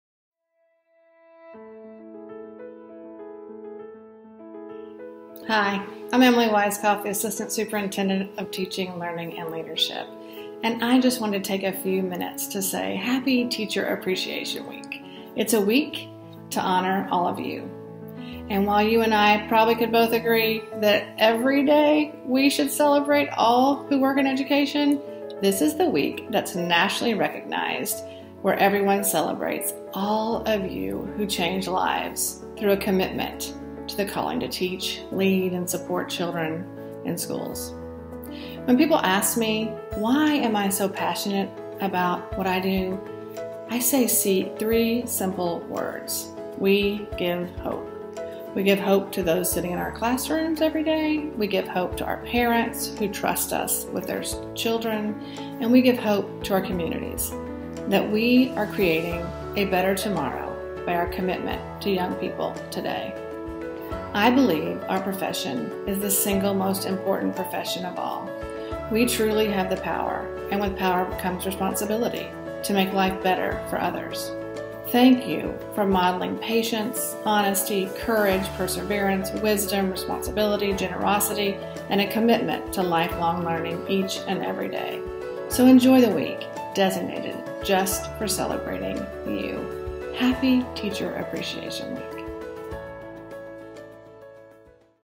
Teacher Appreciation Week: A Message from Emily Weiskopf
Emily Weiskopf, Assistant Superintendent of Teaching, Learning and Leadership has a special message for Lake County Schools teachers in honor of Teacher Appreciation Week.